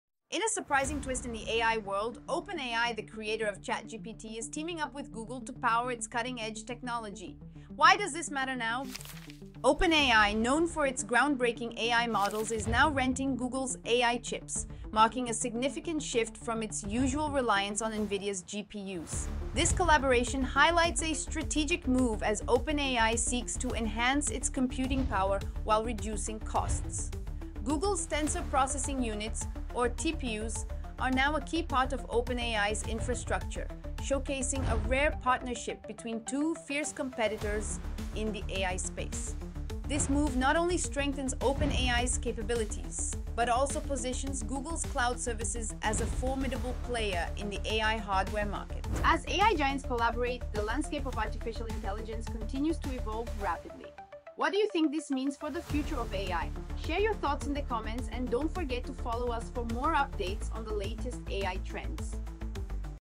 OpenAI Teams Up with Google: A New Era in AI Computing
OpenAI, the innovative force behind ChatGPT, is taking a bold step by partnering with Google to utilize its AI chips, breaking away from its traditional Nvidia GPU dependency. This strategic alliance highlights OpenAI's commitment to boosting its computing power while optimizing costs. Discover how Google's tensor processing units (TPUs) are now integral to OpenAI's infrastructure, marking a significant collaboration between two major AI competitors.

Join us as we explore what this partnership means for the future of AI technology and the competitive landscape.